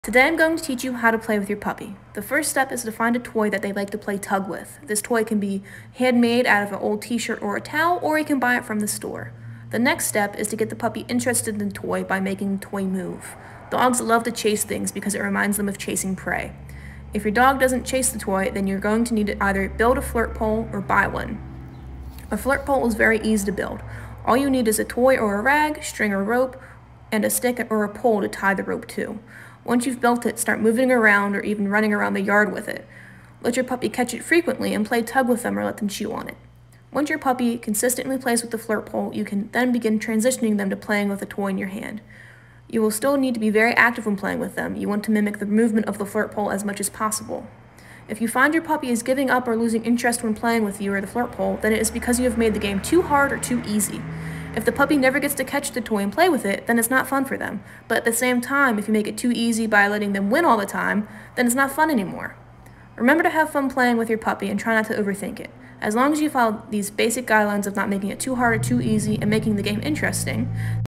How To Play With Your Puppy!
Does your puppy not play with you? Or do they lose interest quick? Here’s a video on how to get them to play! Once your dog plays with you check out our video on how to get them to drop anything!

“There are no bad dogs-we can prove it!”
K9koncepts.com
Sparrows Point, Maryland